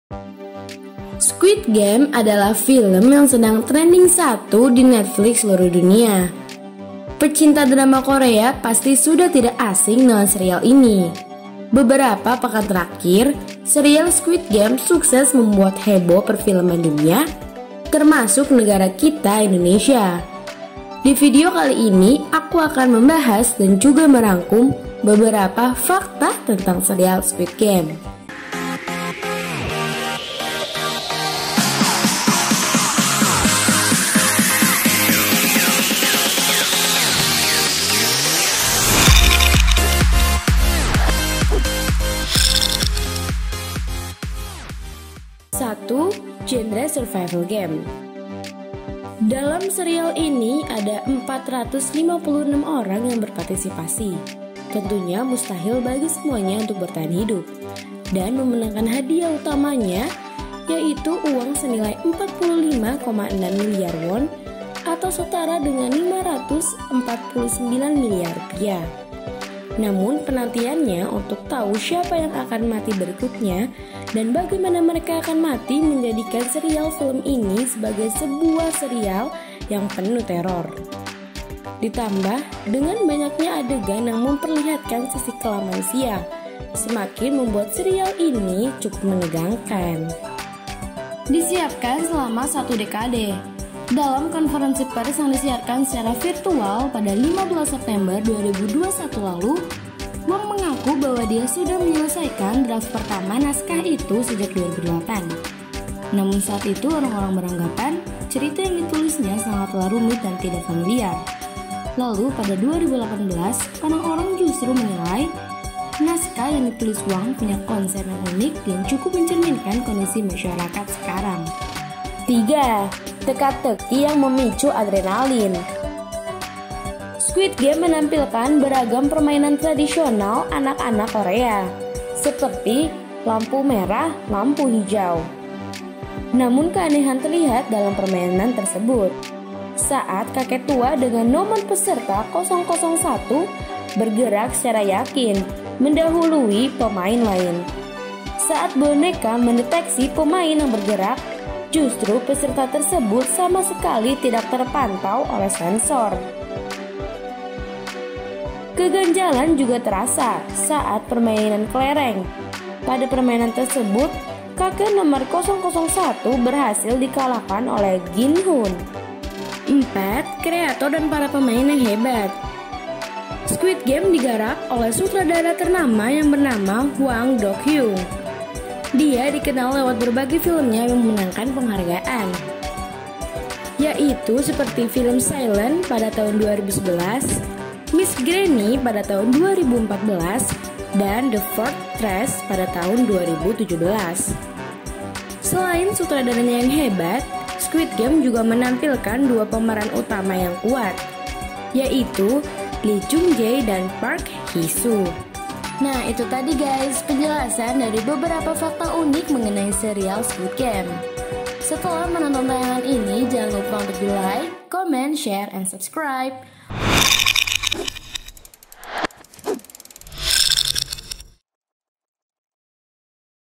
Fakta Unik Squid Game
Squid game adalah film yang sedang Trending 1 di Netflix seluruh dunia, pecinta drama Korea pasti sudah tidak asing dengan serial

 beberapa pekan terakhir, serial Squid Game sukses membuat heboh perfilman dunia, termasuk Megara Kita Indonesia.

Divideo kali ini aku akan membahas dan merangkum beberapa fakta tentang serial squid